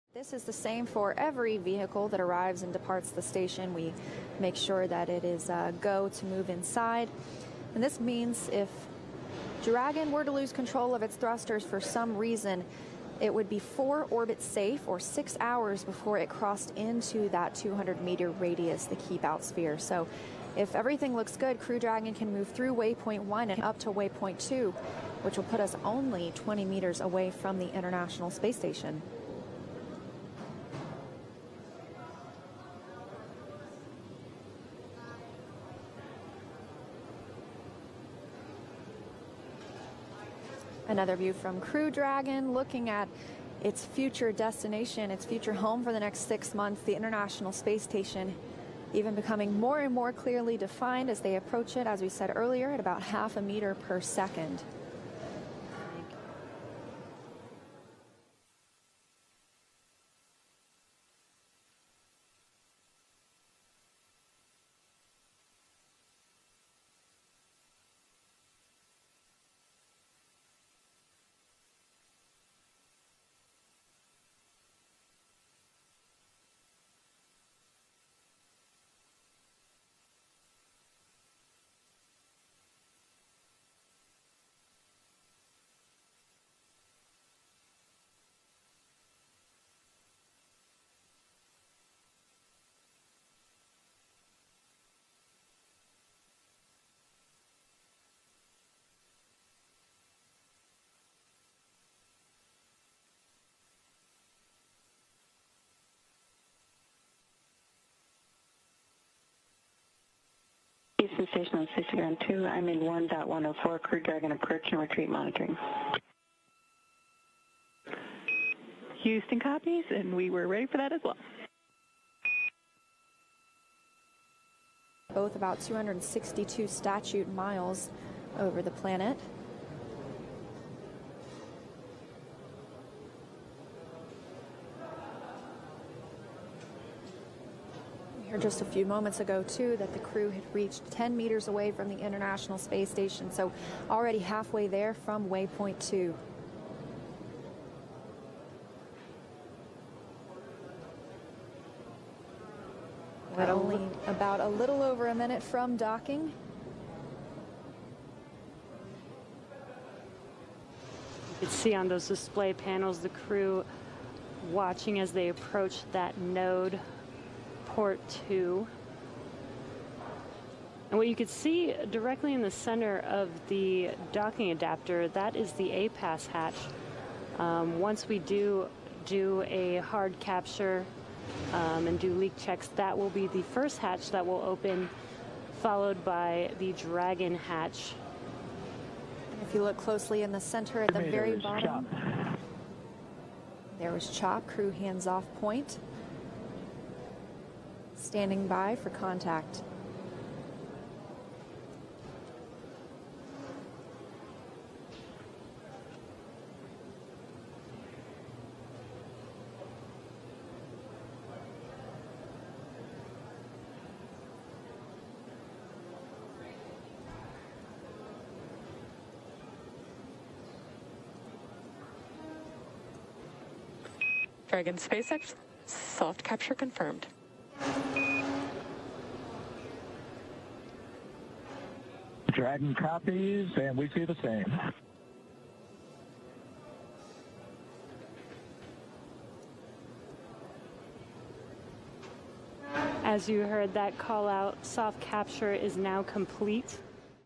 Stunning video captures SpaceX Crew Dragon's approach to ISS
A SpaceX Crew Dragon carrying four astronauts docked with the International Space Station on Monday. It's the first of what NASA hopes will be many routine missions ending a reliance upon Russian rockets. The current inhabitants of the ISS welcome their new crew mates with open arms upon their arrival in jubilant scenes broadcast by NASA. The journey took just over 27 hours as the spacecraft, named 'Resilience' docked autonomously with the space station some 260 miles above Earth. The crew is comprised of three Americans -- Michael Hopkins, Victor Glover and Shannon Walker -- and Japan's Soichi Noguchi.